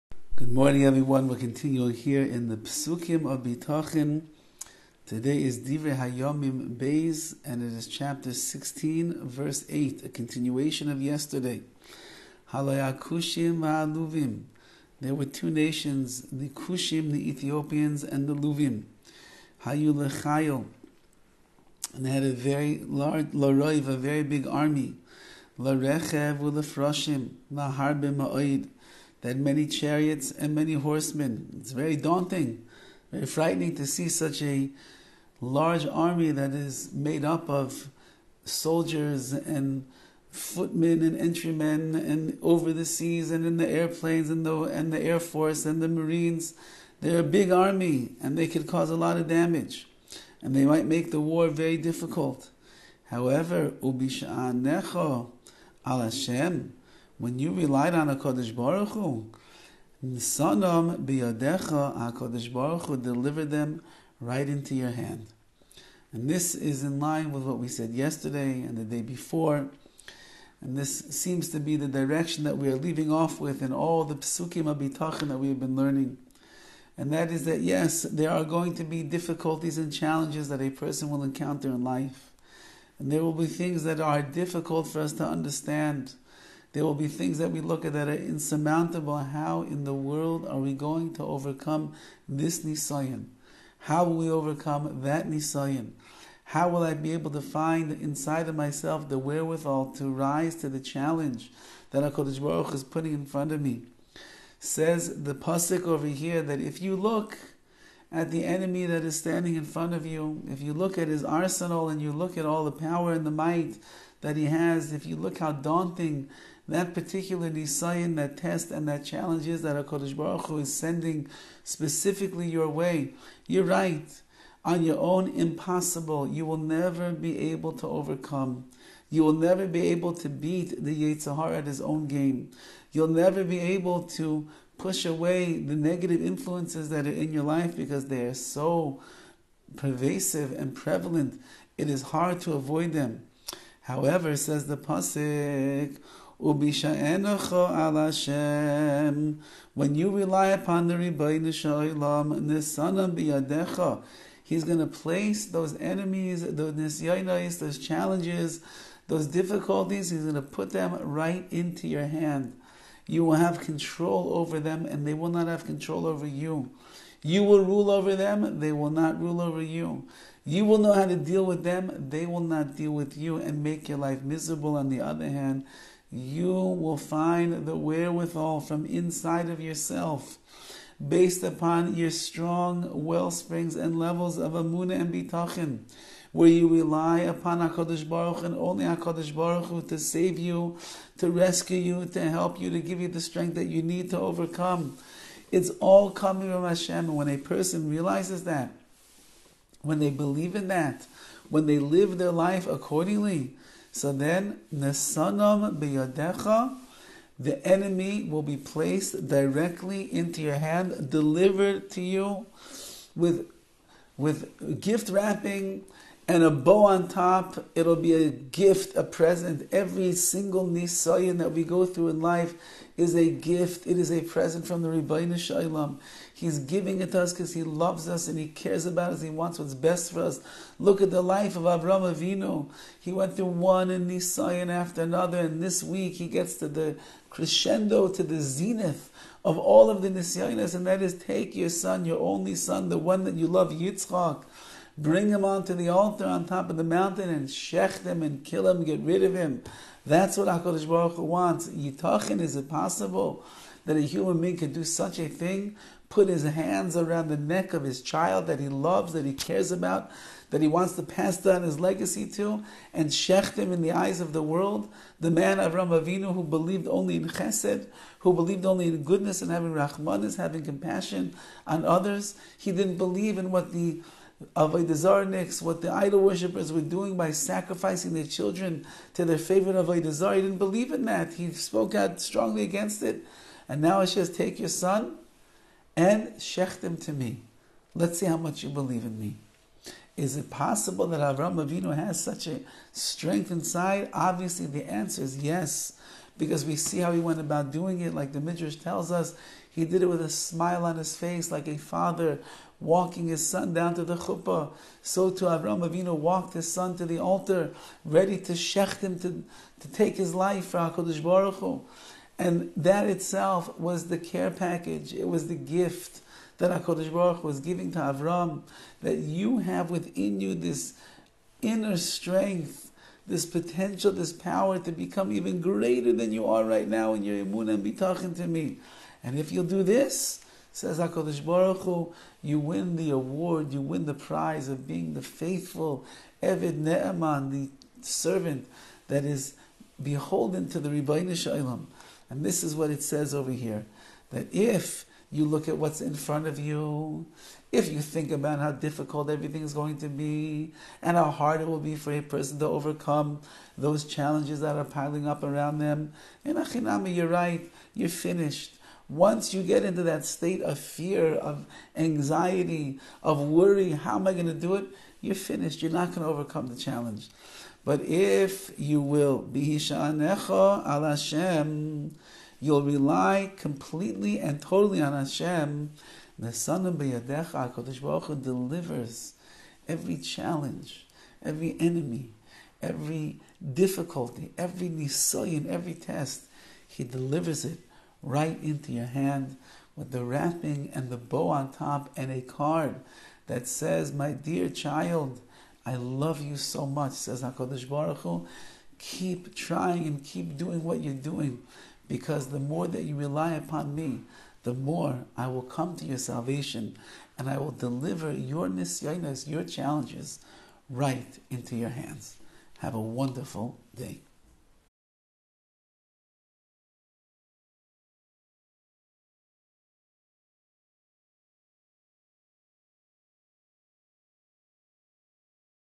Hashem Will Deliver The Salvations Right Into Your Hand - Bitachon #271
Bitachon #271 - Hashem Will Deliver The Salvations Right Into Your Hand